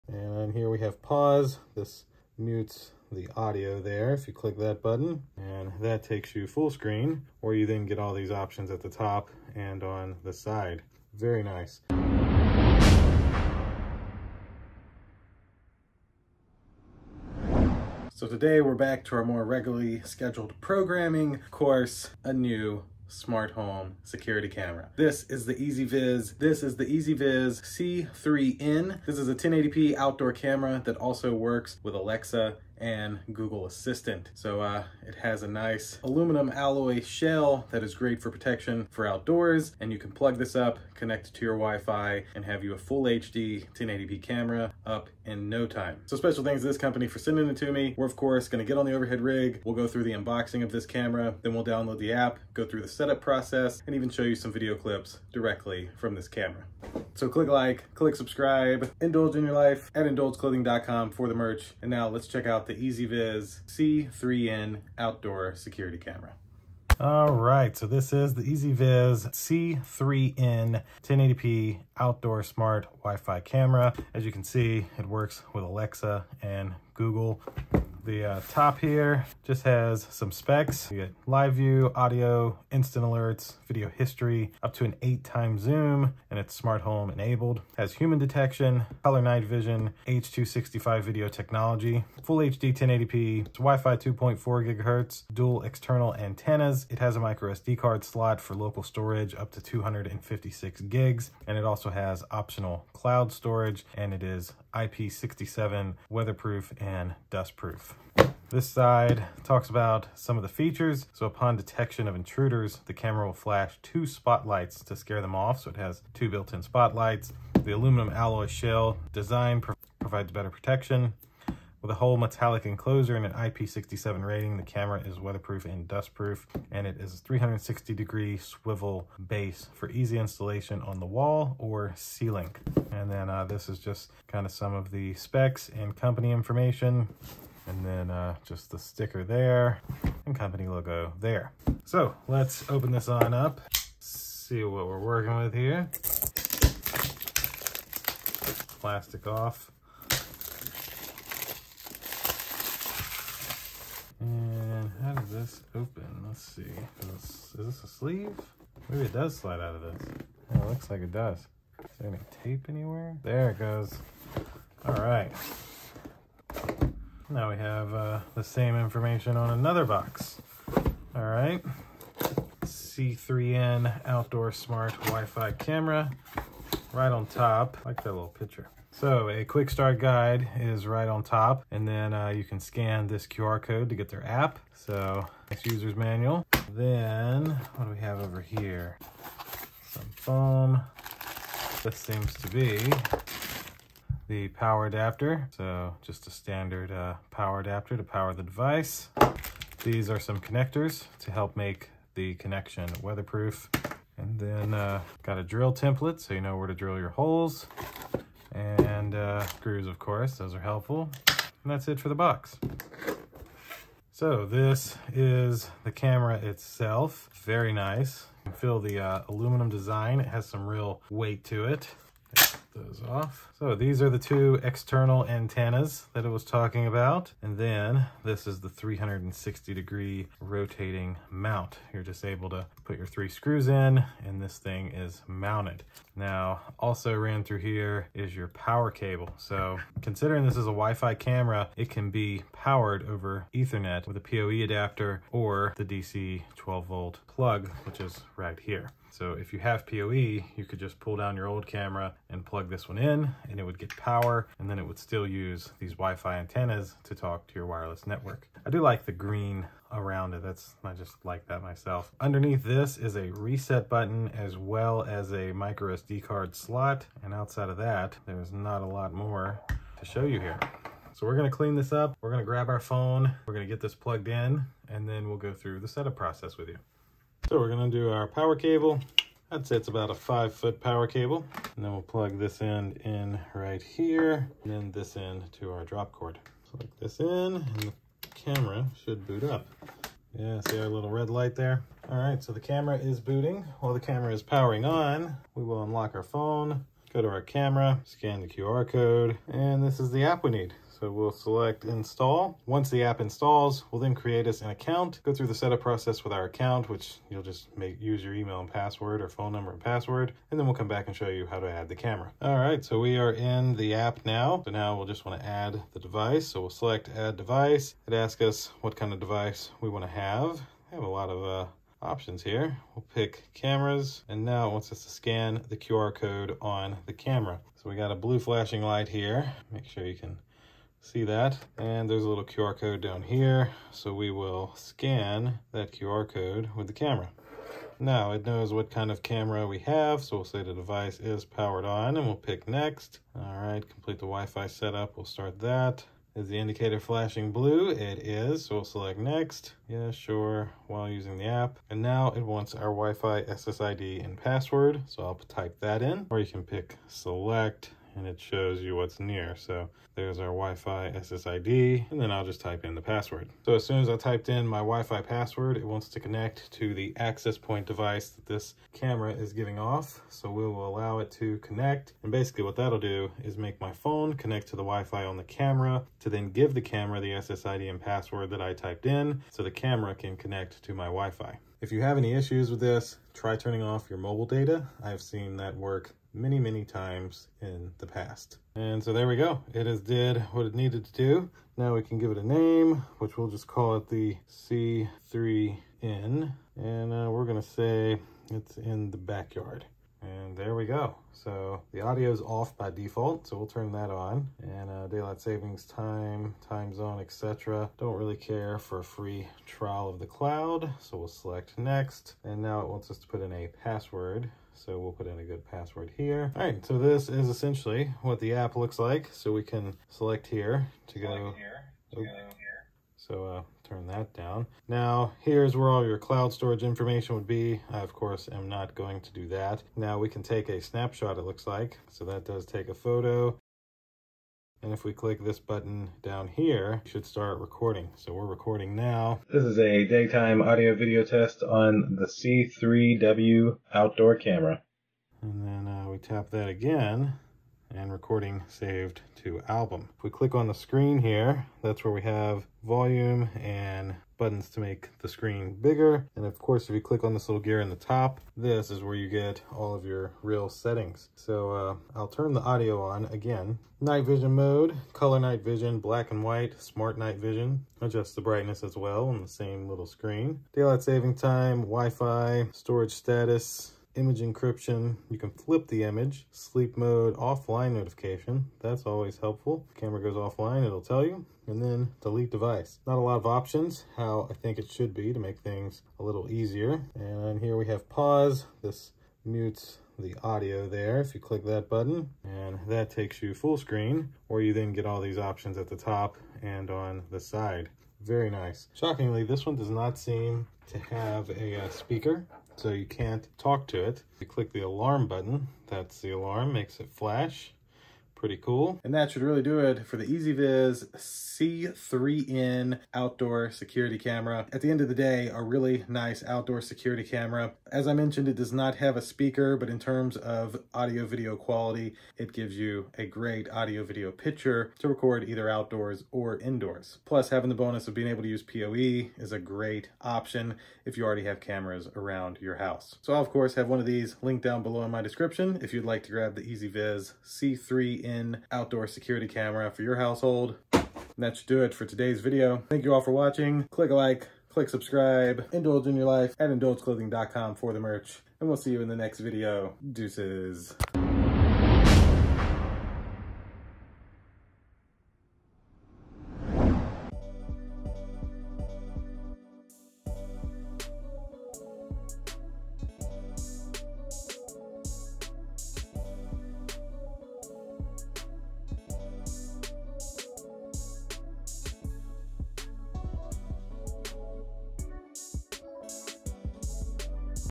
EZVIZ C3N Outdoor Smart Security Camera Setup & Review
Today we unbox and setup the EZVIZ C3N Outdoor Smart WiFi Camera. This is a full HD Outdoor Camera that can be powered by POE and works with Alexa and Google Assistant. See the full setup process in todays video.

Do you want a EZVIZ C3N Outdoor Smart Wi-Fi Camera?